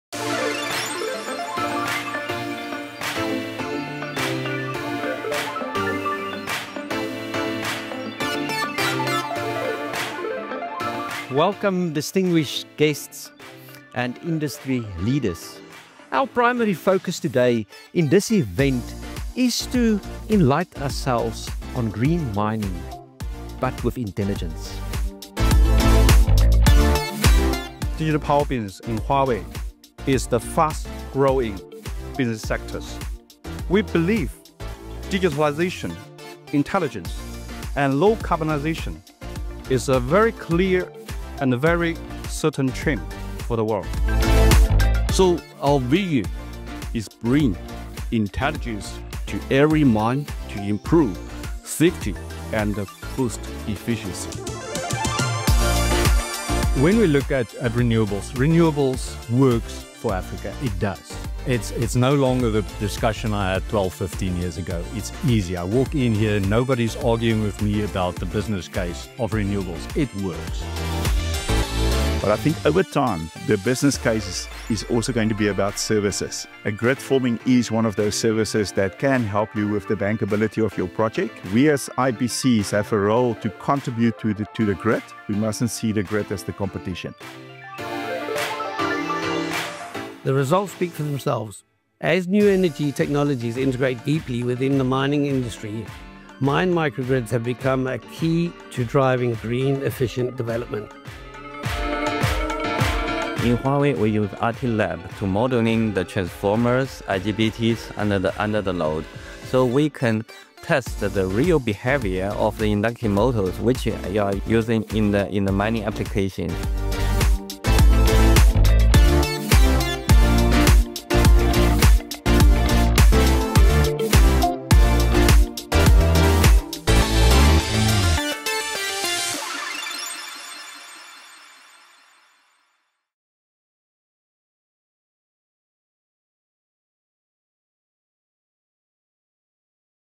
🌍 Lighting Up a Greener Africa with Huawei: Smart and Low-Carbon Mining 🏆
At the recent, Smart & Green Mining: Sustainability & Decarbonisation Summit, we enjoyed insights from our industry, and the voices of industry leaders - all expertly led by Herman Fourie 🚀